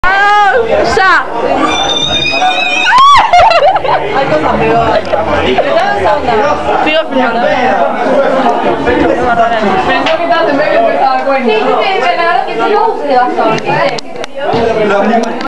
Nati en tirolesa
Nati tirandose en tirolesa luego del tomarse un Jose oro...

(Para los amigos era parapente)